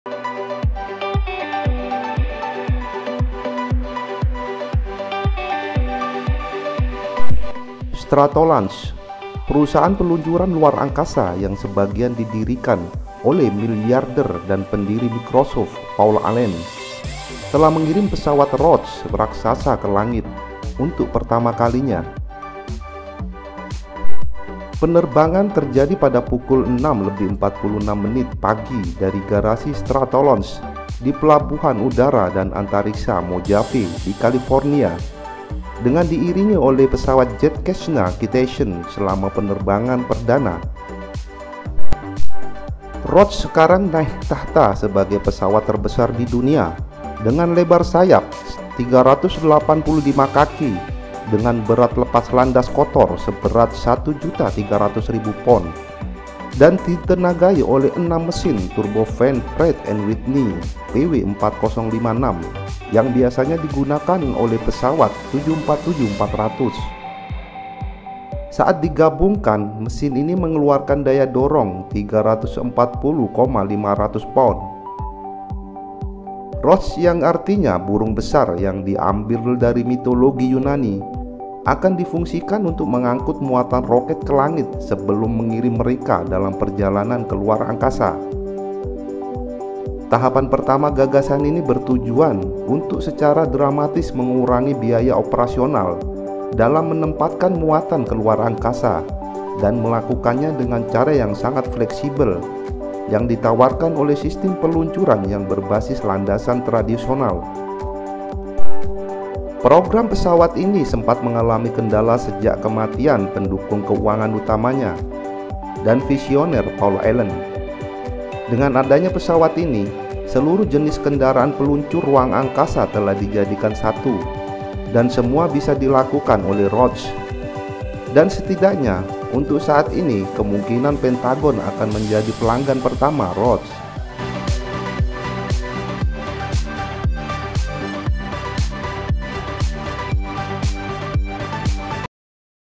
Pesawat terbesar di dunia Stratolaunch laksanakan terbang perdana
Stratolaunch, perusahaan peluncuran luar angkasa yang sebagian didirikan oleh miliarder dan pendiri Microsoft Paul Allen, telah mengirim pesawat Roc raksasa ke langit untuk pertama kalinya. Penerbangan terjadi pada  pukul 6:46 pagi dari garasi  Stratolaunch di Pelabuhan Udara dan Antariksa Mojave di California dengan di iringi oleh jet Cessna Citation selama penerbangan perdana.

Roc sekarang naik takhta sebagai pesawat terbesar di dunia dengan lebar sayap 385 kaki, berat lepas landas kotor seberat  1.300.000 pound, dan ditenagai oleh enam mesin Turbofan Pratt & Whitney PW4056 yang biasanya digunakan  pada pesawat 747-400. saat Digabungkan mesin ini mengeluarkan daya dorong 340.500 pound. Roc yang artinya  burung besar yang diambil dari mitologi Yunani - akan difungsikan untuk  mengangkut muatan roket ke langit sebelum mengirim mereka dalam perjalanan ke luar angkasa.